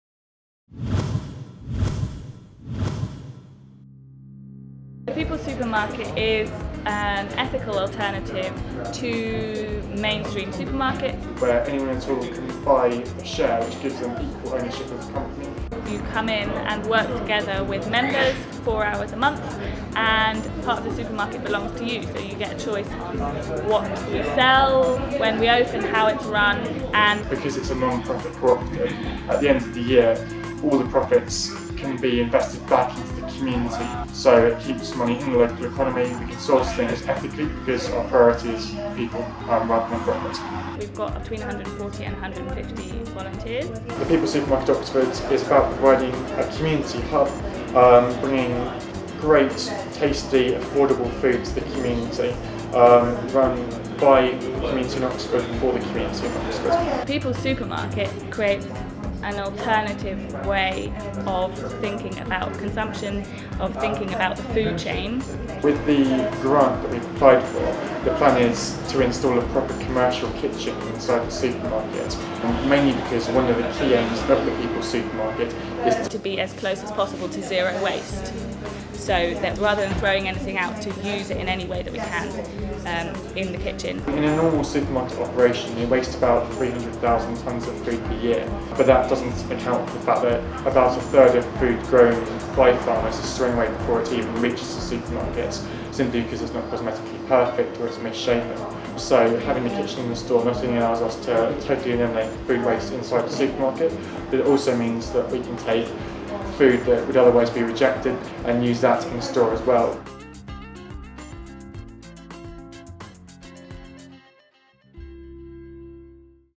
The People's Supermarket Oxford
The People's Supermarket Oxford is a newly opened, community run, owned, and managed co-operative alternative to corporate supermarkets.

This video is part of the Opus Energy Gift Competition - TPS Oxford are applying for funding to put a kitchen into the store so that they can use any food that would otherwise be wasted.